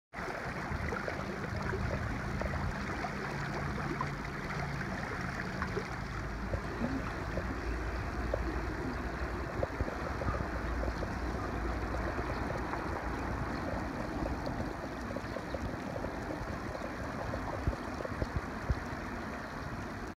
The rivers are flowing again, but it hasn't rained.
Filmed Sunday 2nd April 2017. Temperature 13deg Celsius at 2pm. The rivers are flowing again even though it hasn't rained. Snow is melting, iced up rivers have thawed out but the grass is still brown waiting for some rain.